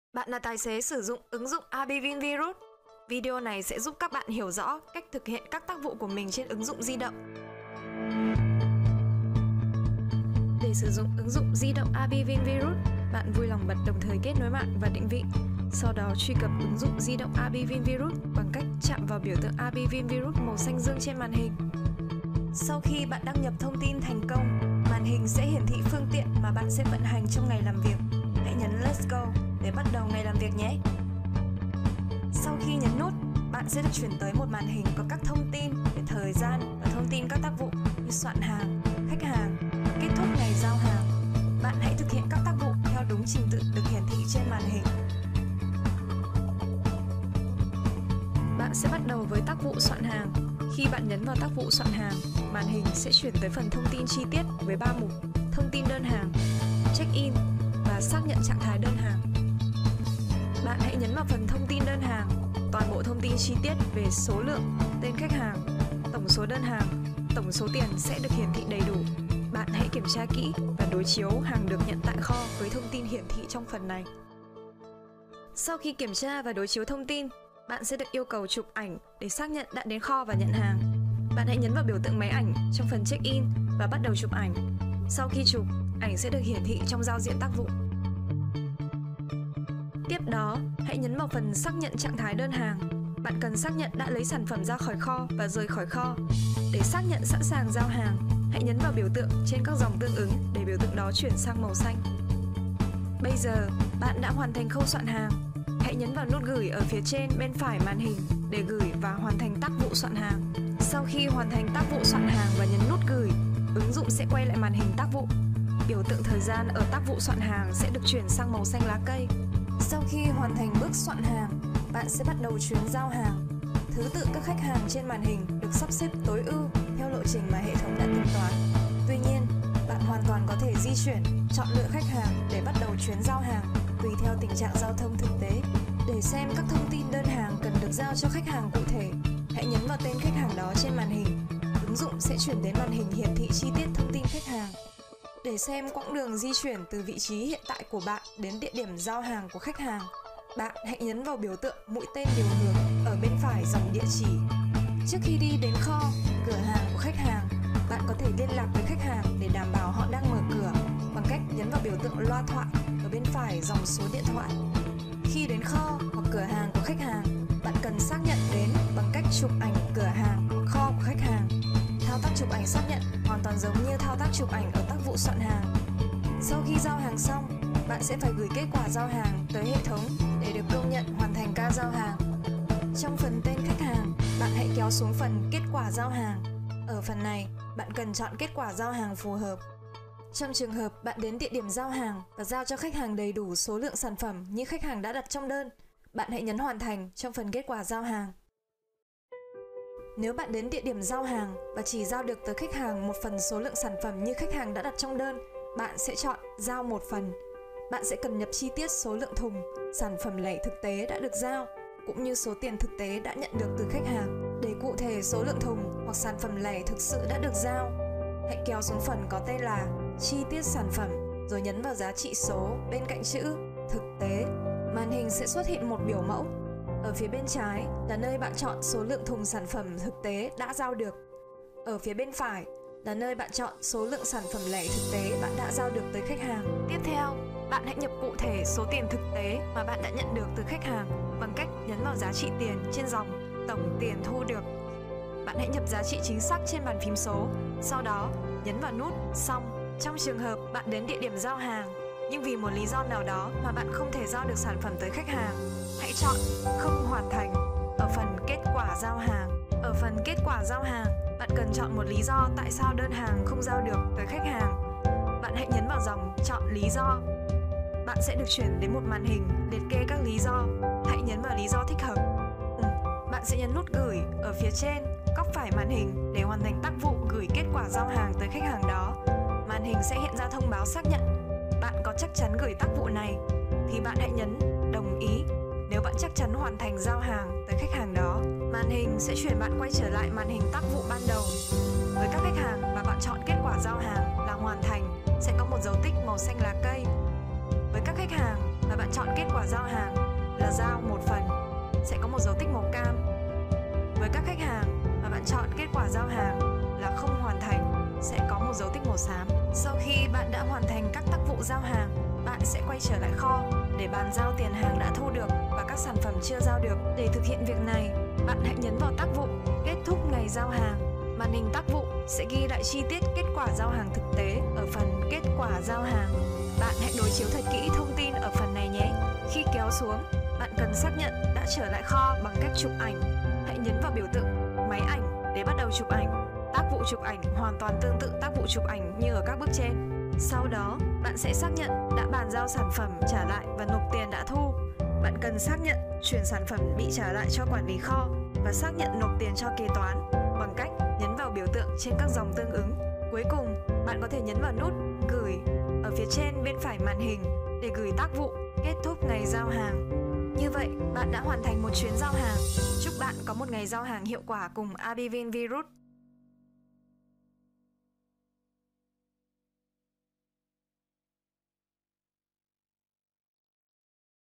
Hệ thống quản lý Vận tải - Abivin vRoute 4.0 - Ứng dụng di động - Tiếng Việt
Abivin vRoute 4.0 là Hệ thống Quản lý Vận tải, cung cấp giải pháp tốt nhất để giảm thiểu chi phí hậu cần, đồng thời tối đa hóa năng suất của đội xe